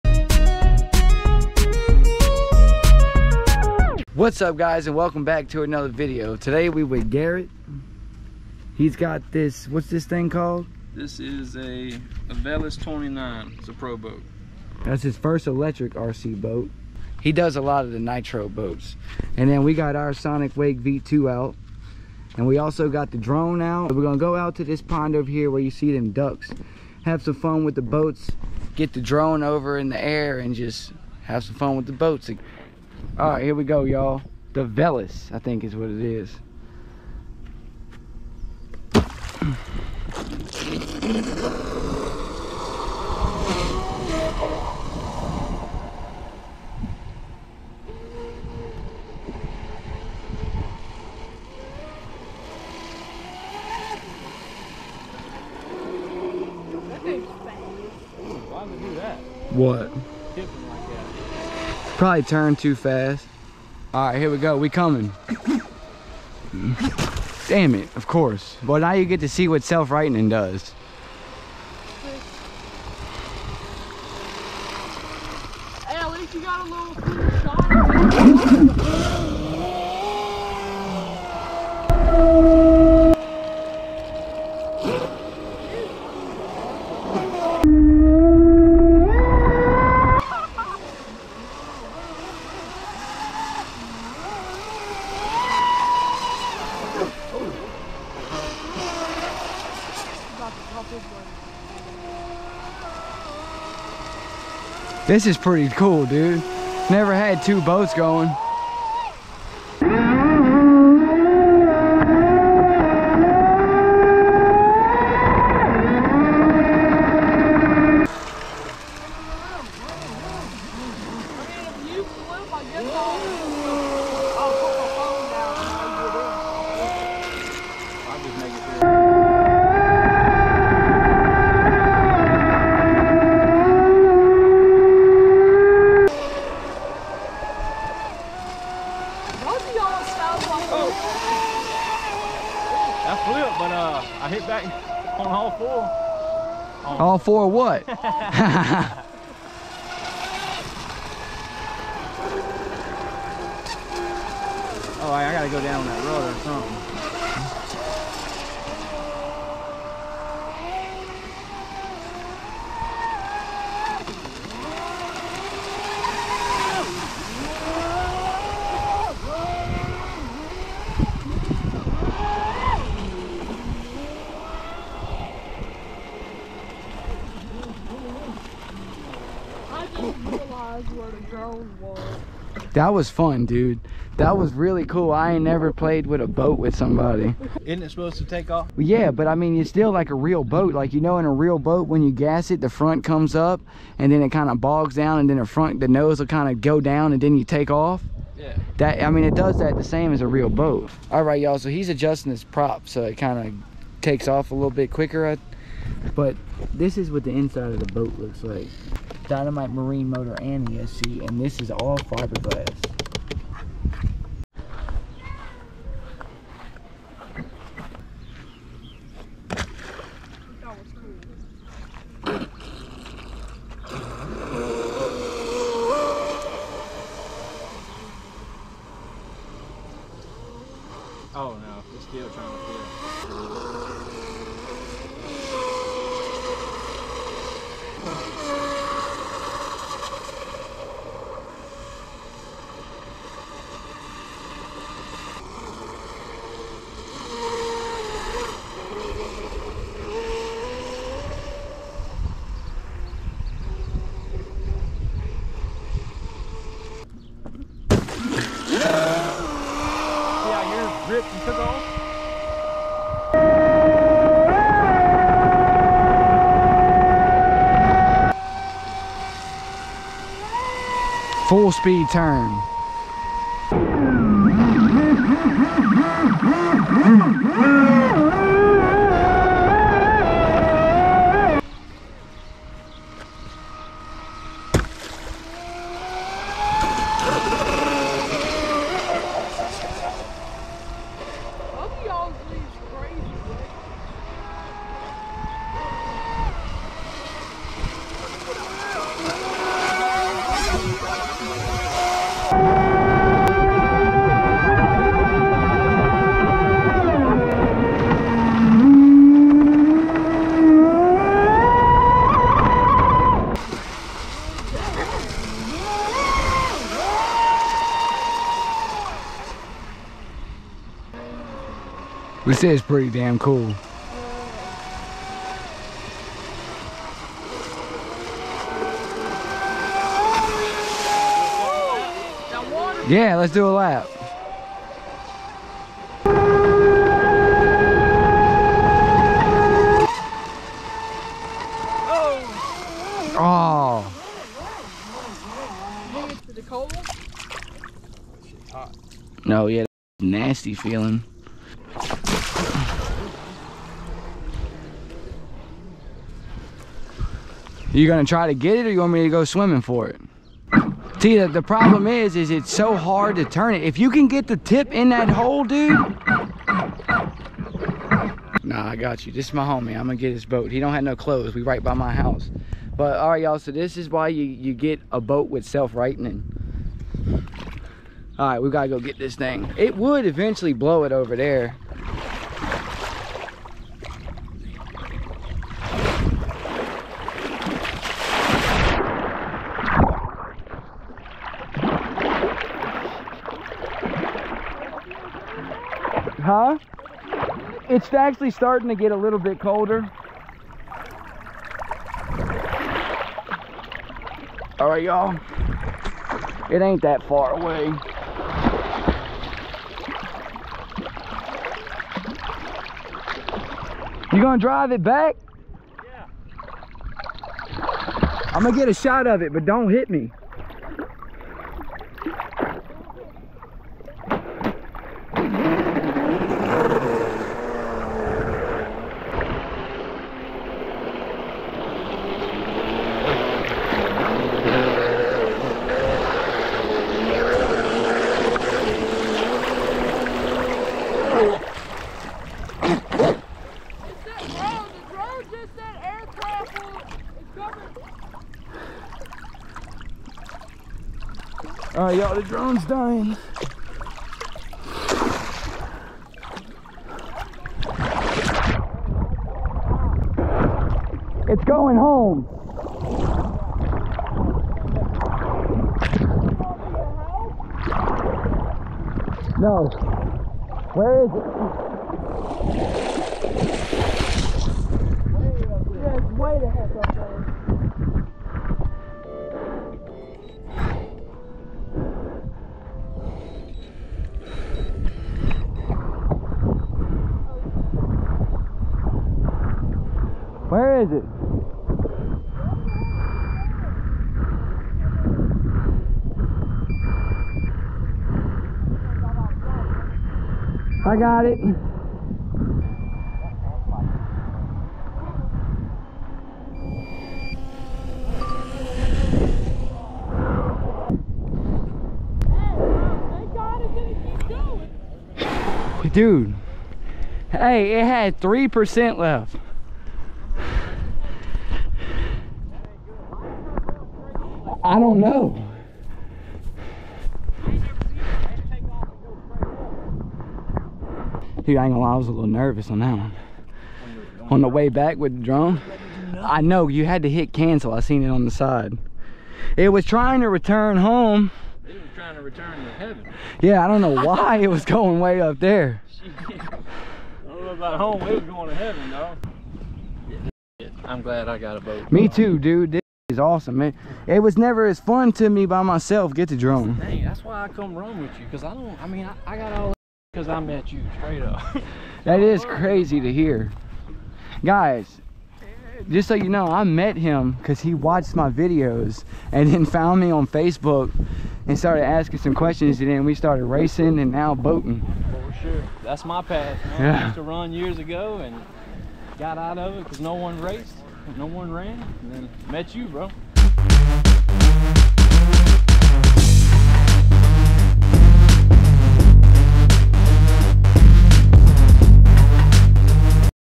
ProBoat Sonic Wake v2 And Veles 29 FULL SPEED Racing In Subdivision Pond
This guy that watches my videos messaged me on Facebook asking me about rc stuff.we got to talking and he bout a rc boat. we went out to the subdivision pod and a blast.

~Brennan Frederick